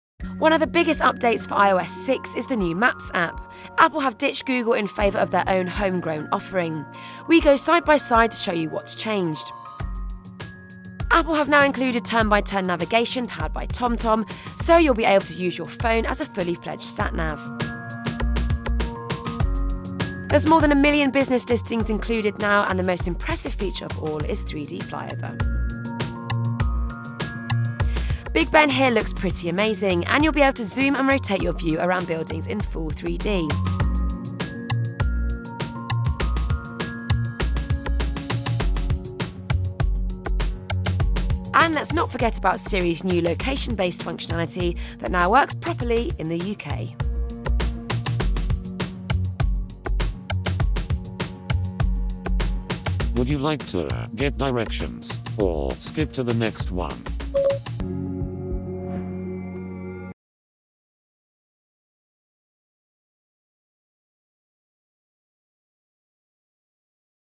iPhone 5 Apple Maps 2012 Preview: iOS 6 Demo & Walk through vs iOS 5 on 4S
Apple Maps Fail? iPhone 5 Release Date 21st September. iOS 6 Release Date: 19th September. iPhone 5 Maps App Preview: iOS 6 Demo & Walk through